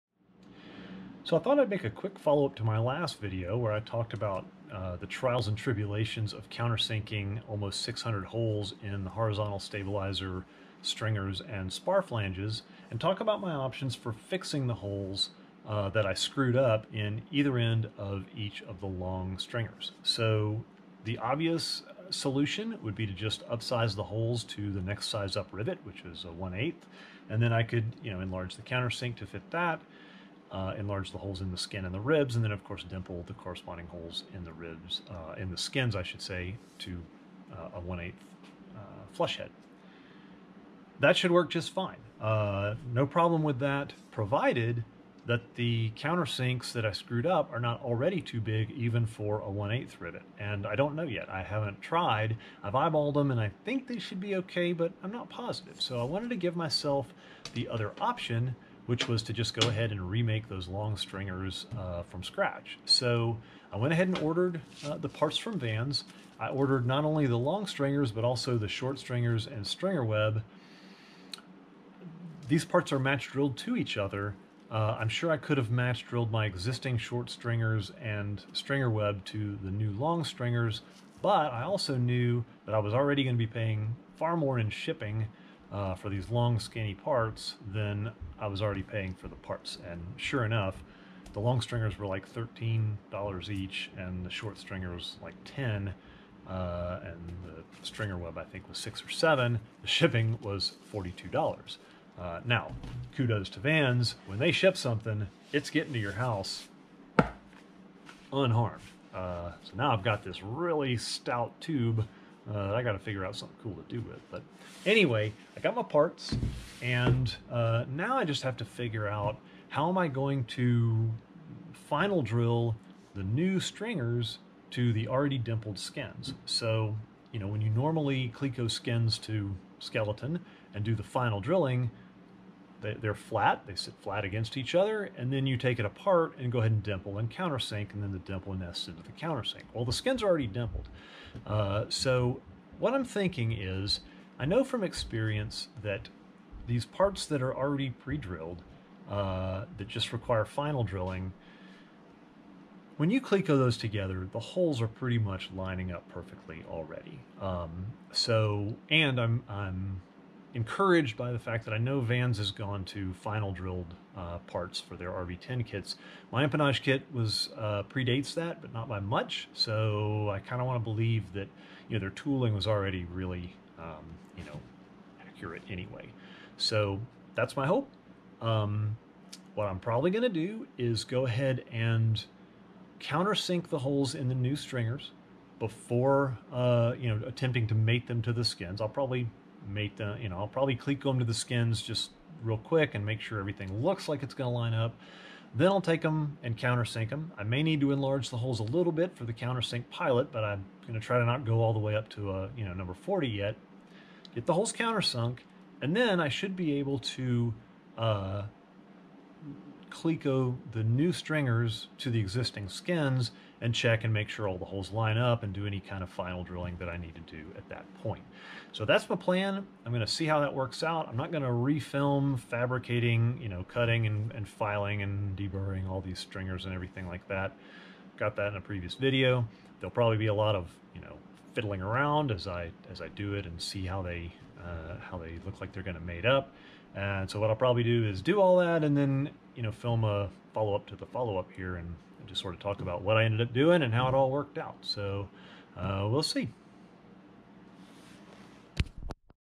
RV-10 Build Log - Emp 46 - Stringer fix options
Quick follow-up to talk about options for fixing the messed up holes on the ends of the HS long stringers.